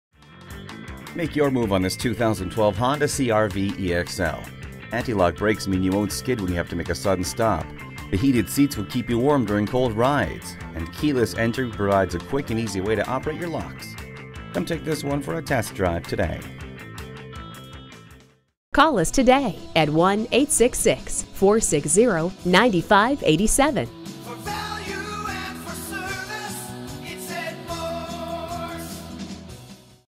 2012 Honda CR-V Brandon FL Lakeland, FL 128392A - SOLD
Year: 2012
Make: Honda
Model: CR-V
Trim: EX-L
Engine: 2.4l 4 Cyl. Fuel Injected
Transmission: FWD Automatic
Color: Dk. Red
Interior: Gray
Mileage: 8973
Stock #: 128392A
Make your move on this 2012 Honda CR-V EX-L. It comes with a 2.40 liter 4 CYL. engine. This is a SUV you can trust - it has a crash test rating of 4 out of 5 stars! Anti-lock brakes mean that you won't skid when you have to make a sudden stop. The heated seats will keep you warm during the cold rides. Keyless entry provides a quick and easy way to operate your locks. According to a review from New Car Test Drive, Controls are functional and for the most part intuitive. Come by today to see this one in person!

Address:
11020 Causeway Blvd